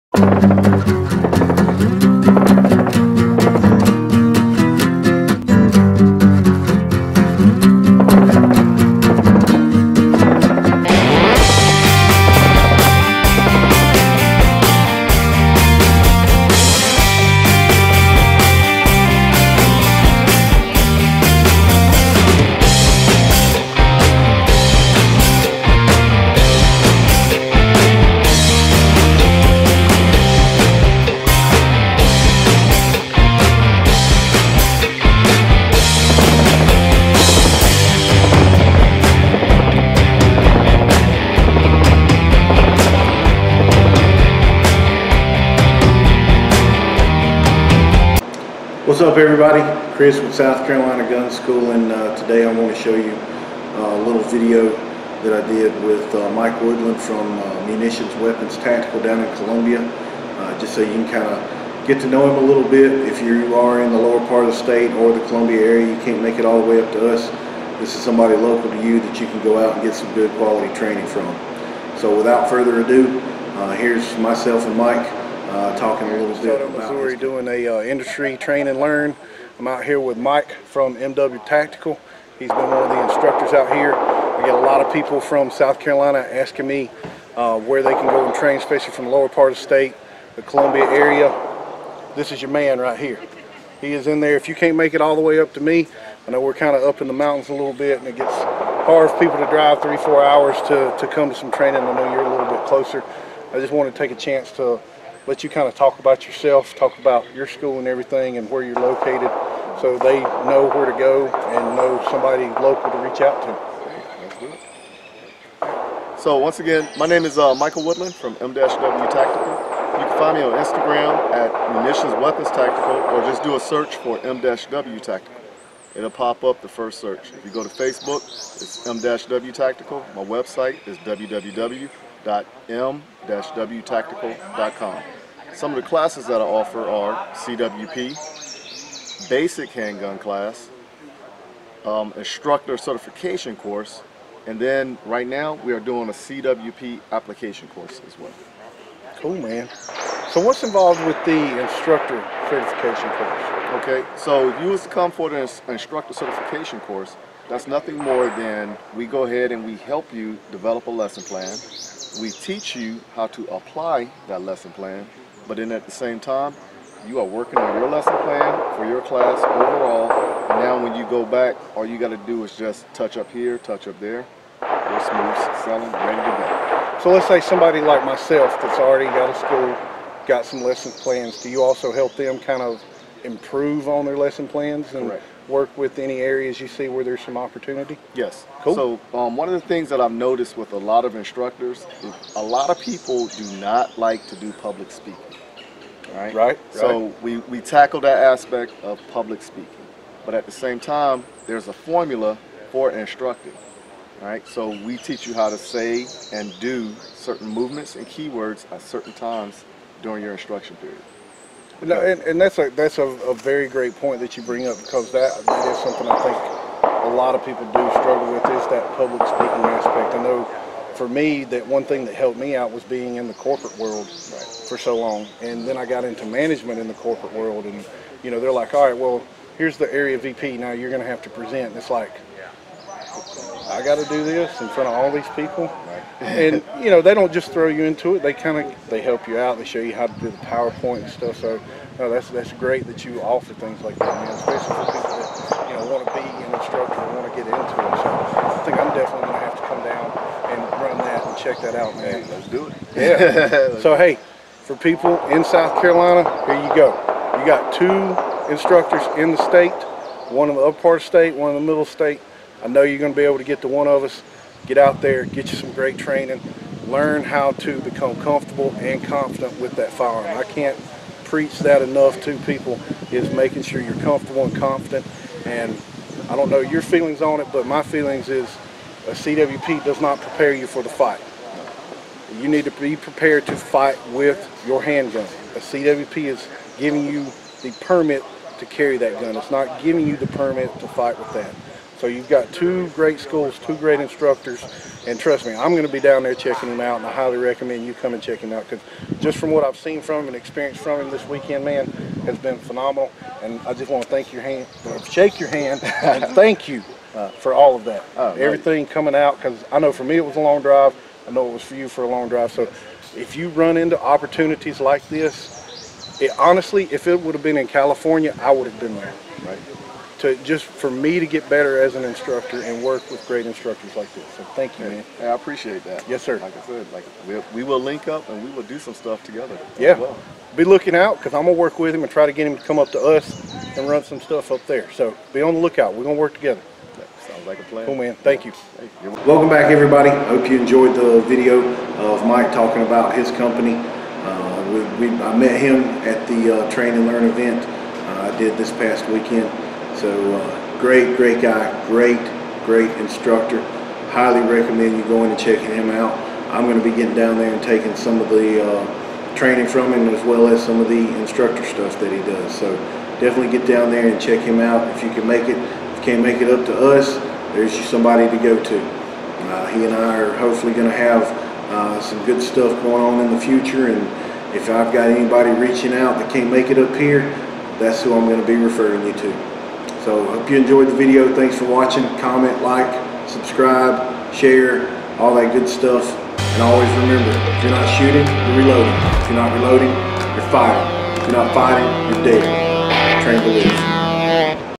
Munitions Weapons Tactical
Here is an interview I did with Michael Woodland from Munitions Weapons Tactical out of Columbia, SC.  We met thru Kevin Dixie with NOC Firearms Training and had a chance to work together at the NOC Industry Train and Learn event a few weeks ago.  He also took time out of his schedule to come up and speak at one of the board meetings on my behalf a few weeks ago that I still cannot thank him enough for.

So if you are in the Columbia area or lower part of the state and can't make it to us here is someone you can go to for some great training.  And if you are looking to be an instructor he has courses for that as well.  Also be on the lookout because I am going to be working to get him up here and do work together again.